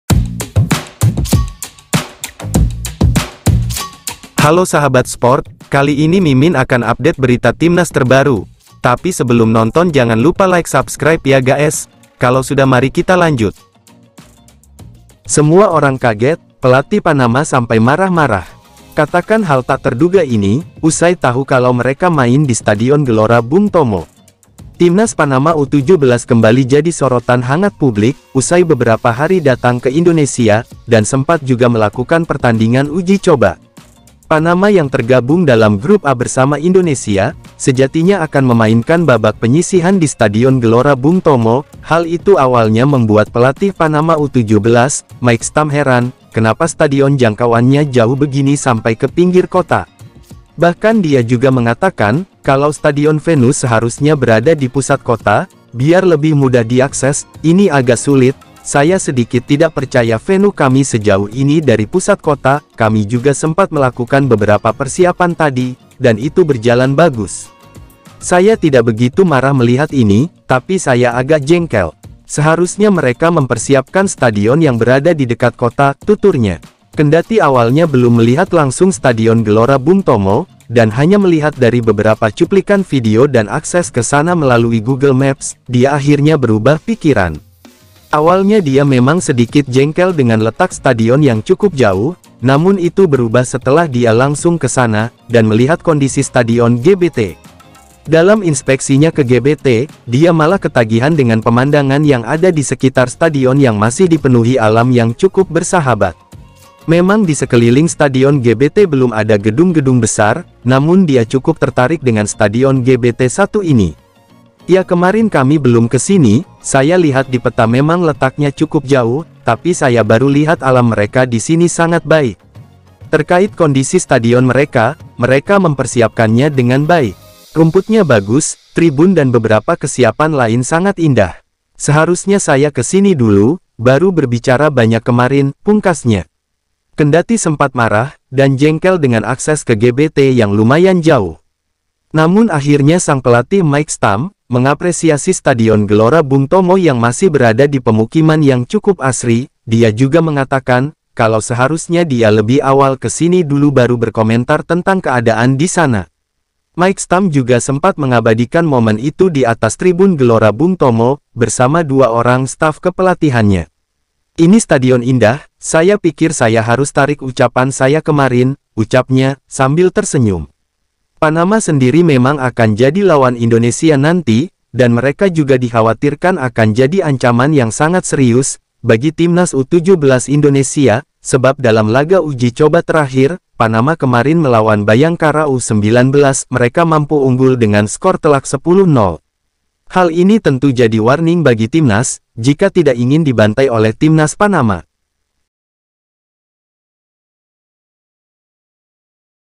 🔴GAWAT PELATIH PANAMA KOK GITU ‼️BERITA TERBARU DARI TIMNAS PANAMA U17 😱😱
semua orang kaget, pelatih Panama sampai marah marah.
 katakan hal tak terduga ini, usai tahu kalau mereka main di Stadion Gelora Bung Tomo. Timnas Panama u17 Kembali jadi sorotan hangat publik. usai beberapa hari datang ke Indonesia, dan sempat juga melakukan pertandingan uji coba.
Panama yang tergabung dalam grup A bersama Indonesia, sejatinya akan memainkan babak penyisihan di Stadion Gelora Bung Tomo. hal itu awalnya membuat pelatih Panama u17,Mike stam heran, kenapa Stadion jangkauannya jauh begini sampai ke pinggir kota. Bahkan dia juga mengatakan, kalau Stadion venu seharusnya berada di pusat kota,biar lebih mudah diakses. ini agak sulit, Saya sedikit tidak percaya venu kami sejauh ini dari pusat kota. kami juga sempat melakukan beberapa persiapan tadi, dan itu berjalan bagus.
saya tidak begitu marah melihat ini, tapi saya Agak jengkel.
 seharusnya mereka mempersiapkan stadion yang berada di dekat Kota,tuturnya. Kendati awalnya belum melihat langsung Stadion Gelora Bung tomo, dan hanya melihat dari beberapa cuplikan video dan akses ke sana melalui Google Maps,dia akhirnya berubah pikiran.
 Awalnya dia memang sedikit jengkel dengan letak stadion yang cukup jauh, namun itu berubah Setelah dia langsung ke sana, dan melihat kondisi Stadion GBT.
 dalam inspeksinya ke gbt,dia malah ketagihan dengan pemandangan yang ada di sekitar stadion yang masih dipenuhi alam yang cukup bersahabat.
memang di sekeliling Stadion GBT belum ada gedung gedung besar, namun dia cukup tertarik dengan Stadion GBT satu ini.
ya Kemarin kami belum ke sini, saya lihat di peta memang letaknya cukup jauh, tapi saya baru lihat Alam Mereka di sini sangat baik.
terkait kondisi Stadion mereka, mereka mempersiapkannya dengan baik.
 rumputnya bagus,Tribun dan beberapa kesiapan lain sangat indah.
 Seharusnya saya ke sini dulu, baru berbicara banyak kemarin, pungkasnya.Kendati sempat marah, dan jengkel dengan akses ke GBT yang lumayan jauh.
namun akhirnya sang pelatih Mike stam,mengapresiasi Stadion Gelora Bung Tomo yang masih berada di pemukiman yang cukup Asri. dia juga mengatakan, kalau seharusnya dia lebih awal ke sini dulu baru berkomentar tentang keadaan di sana.
Mike stam juga sempat mengabadikan momen itu di atas Tribun Gelora Bung Tomo, bersama dua orang staf ke pelatihannya.
ini Stadion indah, saya pikir saya harus tarik ucapan saya kemarin, ucapnya,sambil tersenyum.
 Panama sendiri Memang akan jadi lawan Indonesia nanti,dan mereka juga dikhawatirkan akan jadi ancaman yang sangat serius, bagi Timnas u17 Indonesia. sebab dalam laga uji coba terakhir,Panama kemarin melawan Bayangkara u19. mereka mampu unggul dengan skor telak 10 0.
Hal ini tentu jadi warning bagi Timnas, jika tidak ingin dibantai oleh Timnas  Panama.